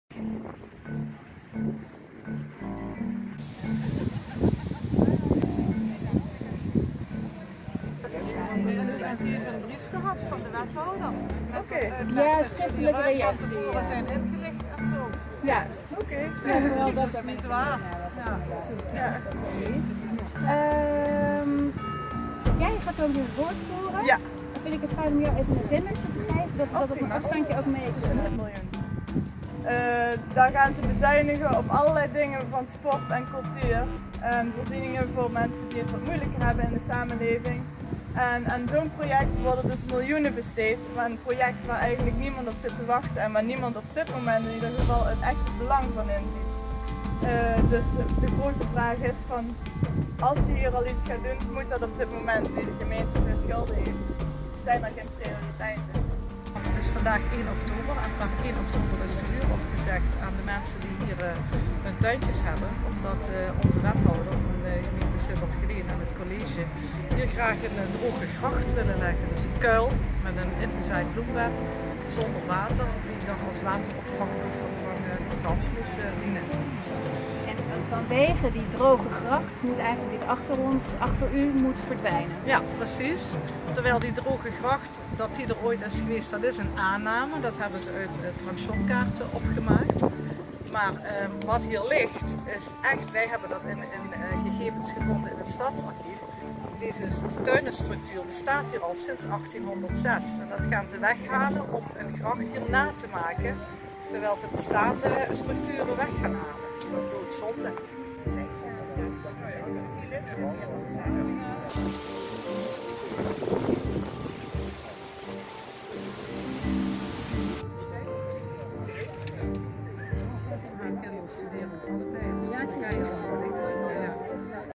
Actie Red de Tuinen 1 oktober
Een korte impressie van de actiedag op 1 oktober van Red de Tuinen. Het SBS6 televisieprogramma Hart van Nederland was ook aanwezig voor een verslag.

Woordvoerder Anique Hoekstra gaf tekst en uitleg over de actie en Nancy Kaufmann van de Stadspartij voor Sittard, Geleen en Born, werd geïnterviewd over de nieuwe bevindingen met betrekking tot de historie die de Stadspartij in de stadsarchieven ontdekte.